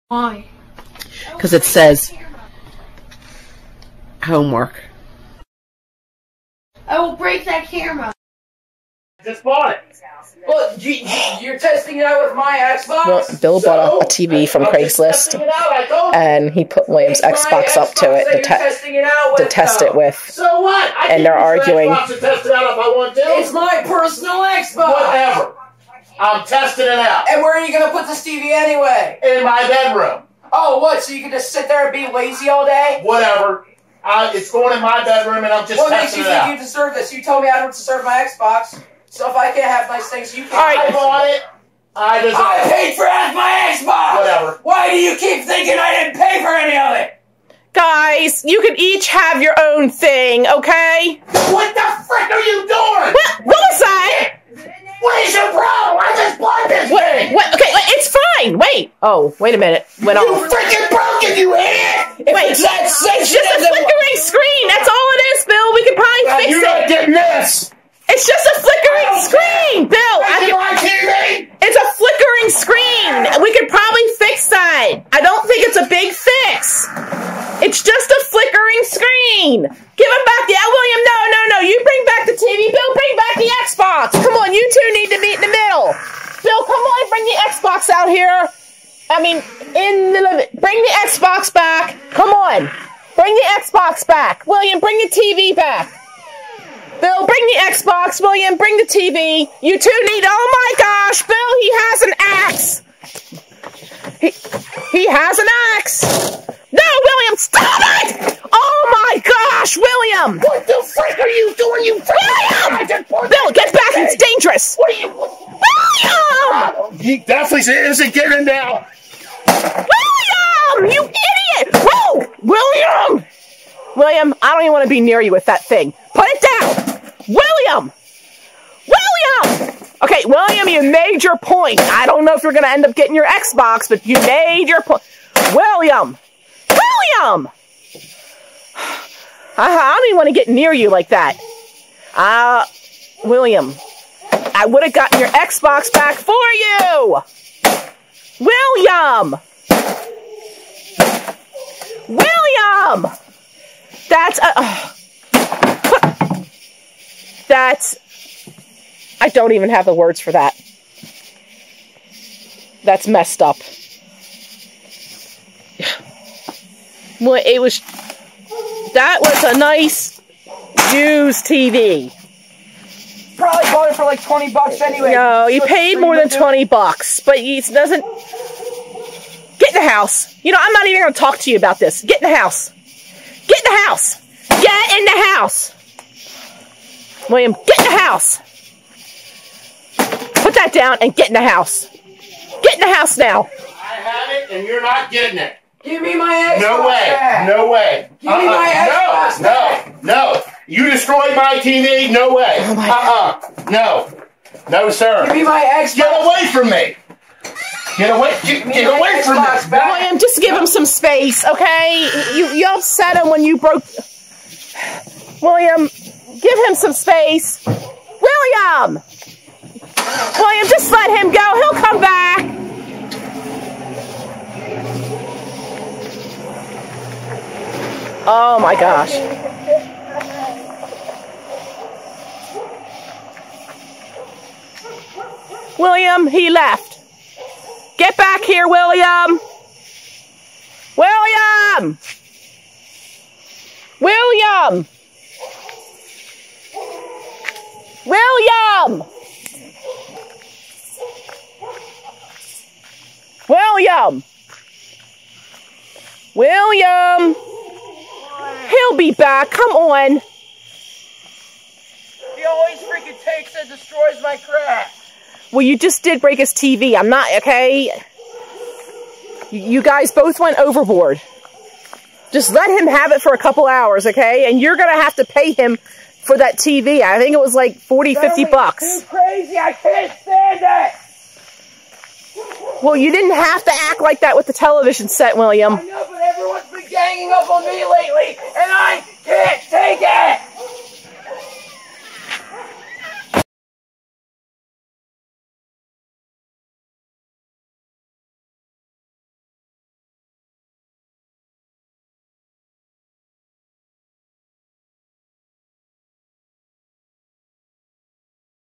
KID AXES DAD'S TV OVER XBOX!!!
William goes crazy and axes his fathers new television after they disagree over his Xbox use.

William's Xbox gamertag is violette1st

Do you want to send mail to any of us at violette1st?  You can send it here:

violette1st
505 Windy Knoll Dr
Unit #1030
Mount Airy, MD 21771
United States